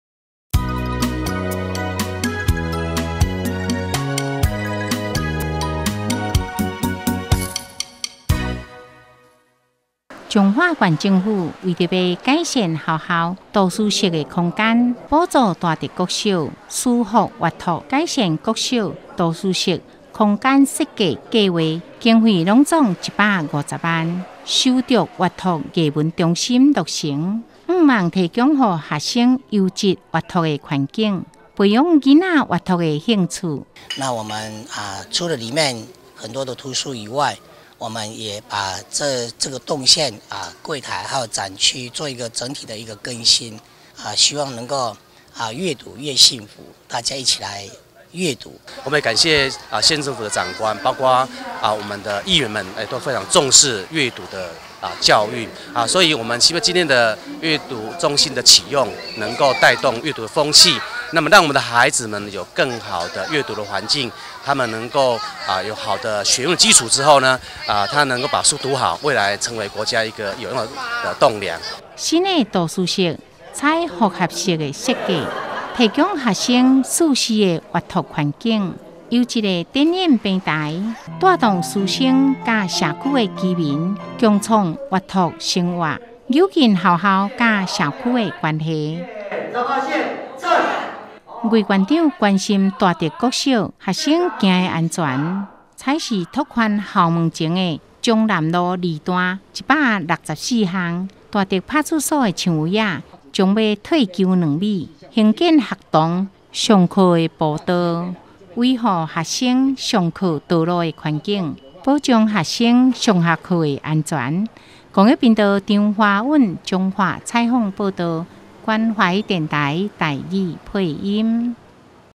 106-05-11 大竹國小百年校慶 閱讀中心啟用、校門道路拓寬(台語版)
彰化縣政府為改善學校圖書室空間，補助大竹國小「書福悅讀」改善國小圖書室空間環境計畫經費共計150萬元，修竹閱讀藝文中心落成啟用。期望提供學童優質的閱讀環境，培養孩子閱讀的興趣。
新圖書室採複合式設計，提供學生舒適的閱讀環境、優質的展演平台，帶動師生與社區民眾共創閱讀生活，拉近學校與社區的關係。
魏縣長關心大竹國小學童行的安全，裁示拓寬校門前的彰南路2段164巷，大竹派出所圍牆將退縮兩米，興建學童通學步道，維護學童上學道路環境，保障學童上下學安全。